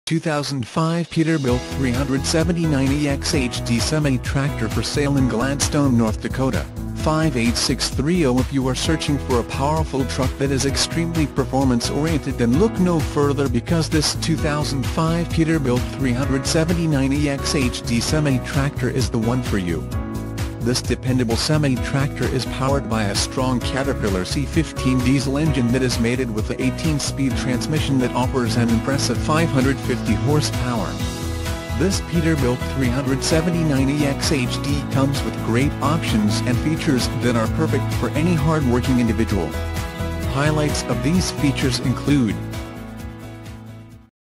2005 Peterbilt 379 GK4917RJ221
2005 Peterbilt 379 EXHD

2005 Peterbilt 379 EXHD Semi Tractor For Sale in Gladstone, North Dakota 58630If you are searching for a powerful truck that is extremely performance oriented then look no further because this 2005 Peterbilt 379 EXHD Semi Tractor is the one for you. This dependable semi tractor is powered by a strong Caterpillar C-15 diesel engine that is mated with a 18-speed transmission that offers an impressive 550 horsepower. This Peterbilt 379 EXHD comes with great options and features that are perfect for any hardworking individual.Highlights of these features include:/font